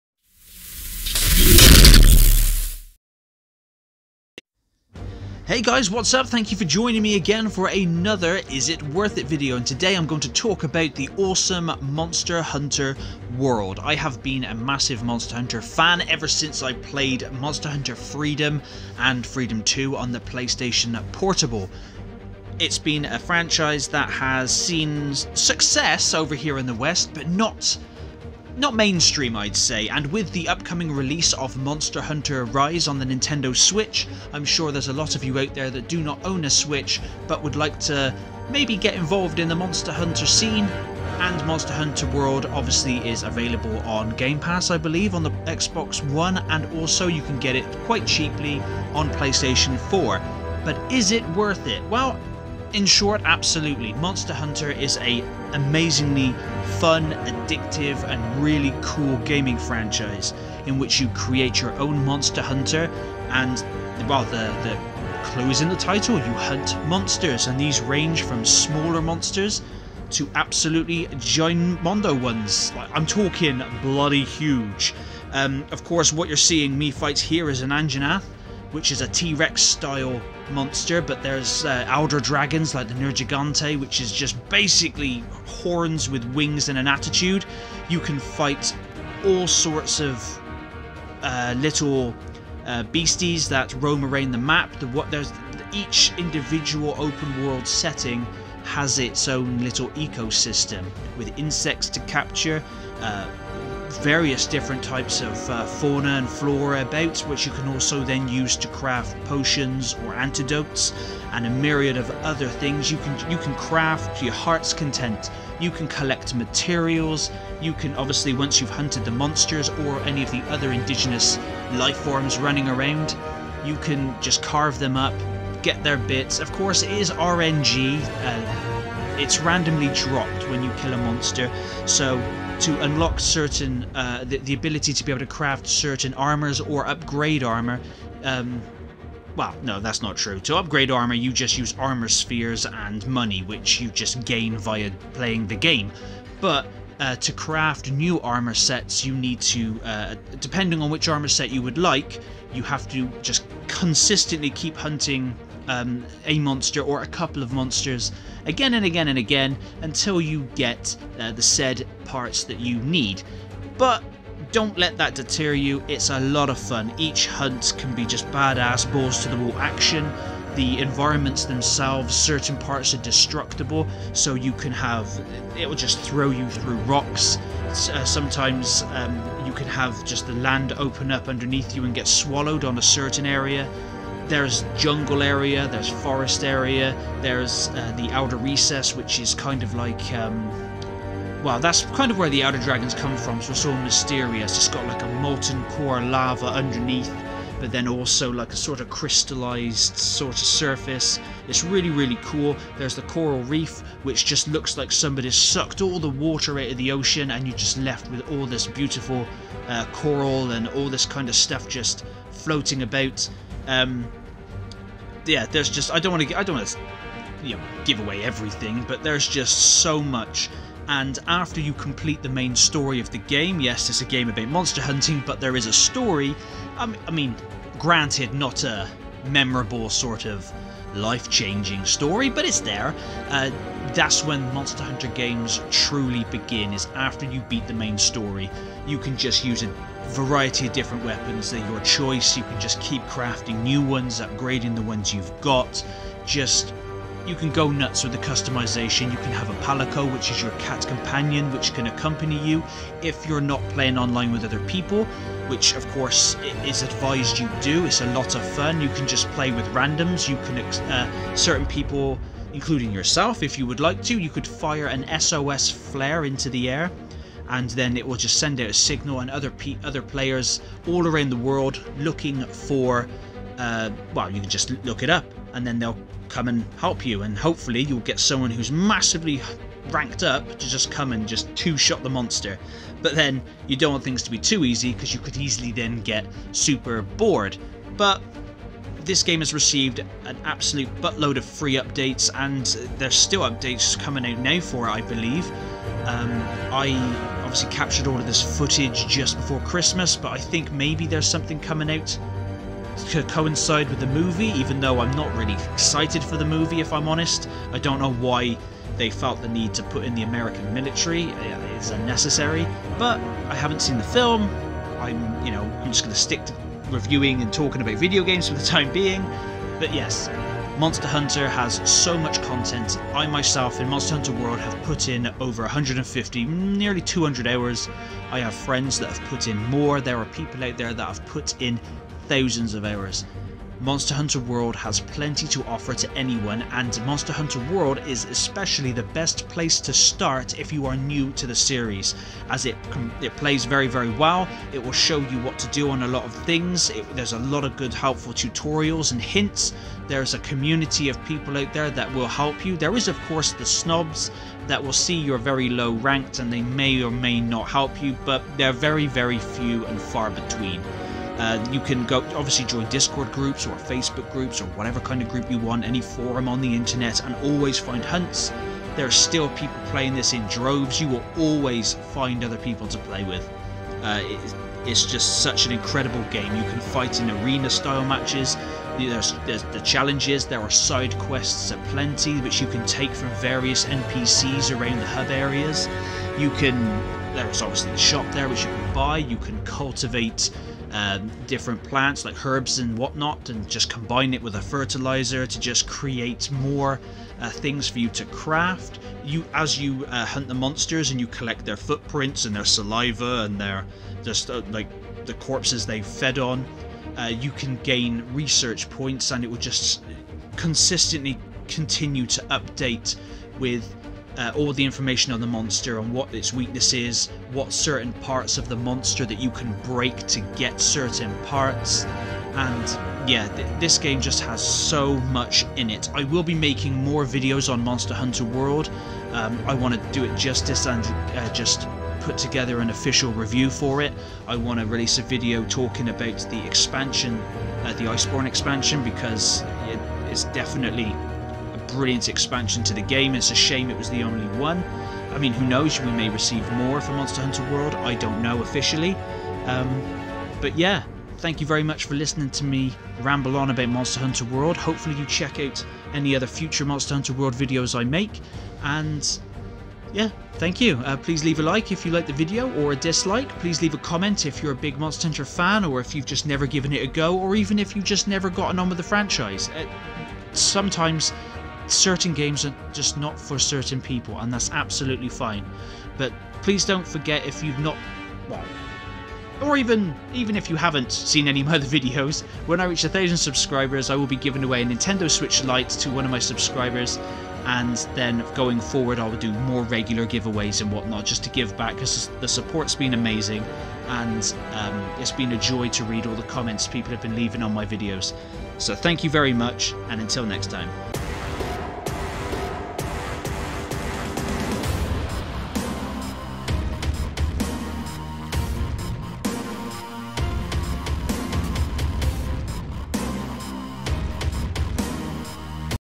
Is Monster Hunter World Worth It In 2021? - MinusInfernoGaming
Hey Guys

Here's my video on whether Monster Hunter World is worth it or not in 2021, hope you enjoy.